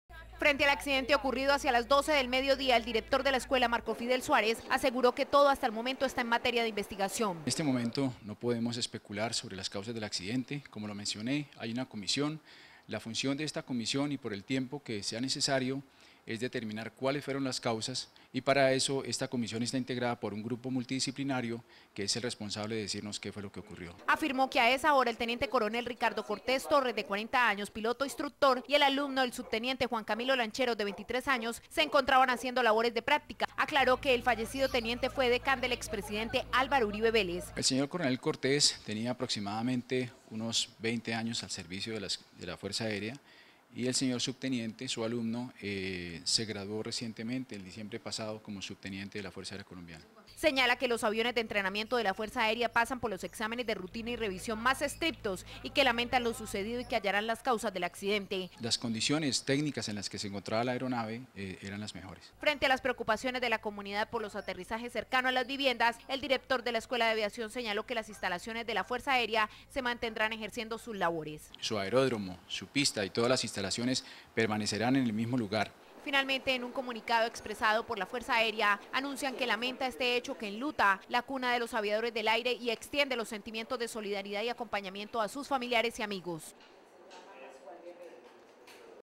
Nota Informativa "Causas del accidente de la avioneta son materia de investigación".
Nota informativa sobre declaraciones de Director de la Escuela Marco Fidel Suarez sobre accidente de avioneta quien afirma que no se puede especular aún sobre causas de accidente.